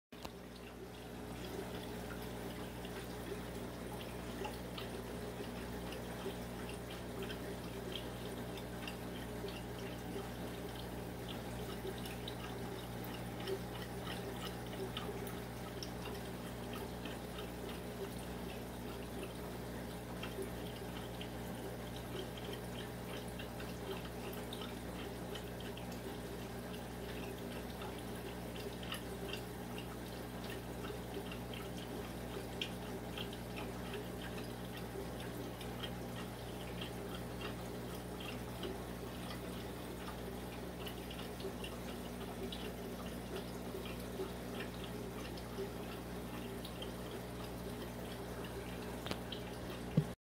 Arulius Barbs swimming together (2022)
Arulius Barbs swimming together!  I have seen these Barbs, but never kept them. Loving them so far!!